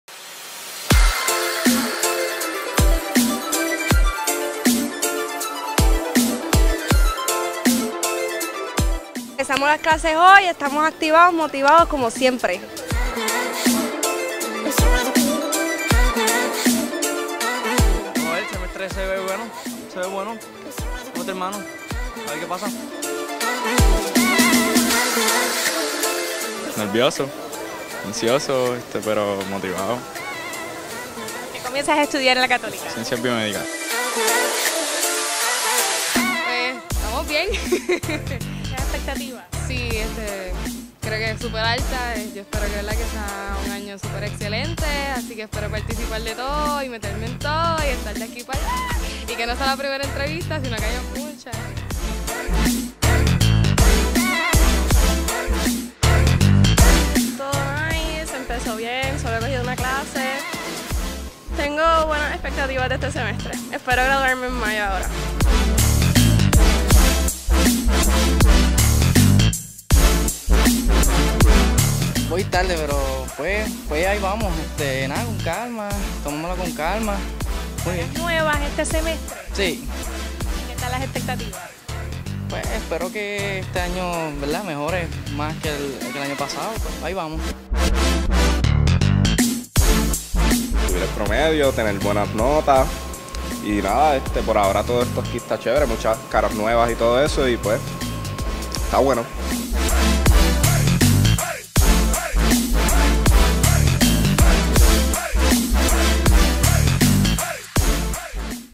¡Bienvenidos Pioneros!
¡Bienvenidos Pioneros! Arranca el semestre académico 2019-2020 y ya los Pioneros están listos.  La Pontificia Universidad Católica de Puerto Rico les dio la bienvenida a todos que inician su jornada académica.  Escucha sus reacciones.